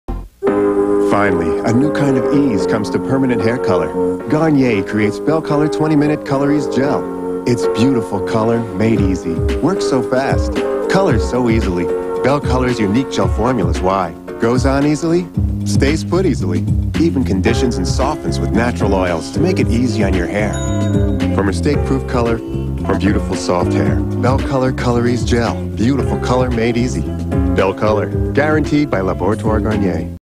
Garnier Belle Color commercial from 1997
Pulled from VHS tape of September 25th 1997 broadcast of CFMT Toronto